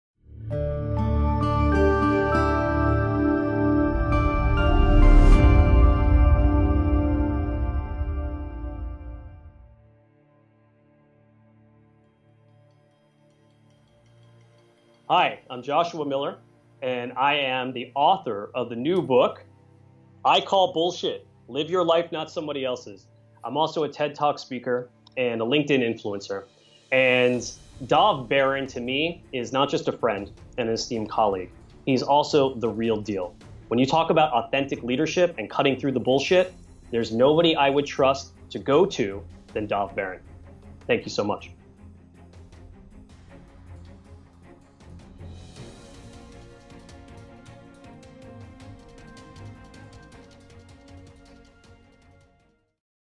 Joshua Miller @coachjhm Creative Director for Fortune 100 & executive coach reviews
Joshua Miller is the Bestselling author of "I Call Bullshit!"
He's a master certified personal & executive coach who spends his life supporting people to bury their bullshit and uncover the greatness they already have.

He started his career as a Creative Director in advertising, responsible for the campaign strategies of Fortune 100 brands, and now creates and supports executive leadership development for many of the same companies.

He’s a TEDx speaker, a Linkedin Top Influencer, a contributing writer for major Industry related journals like Thrive Global and Medium, and creator of “MyInstantCoach”, the first life-coaching app for the iPhone.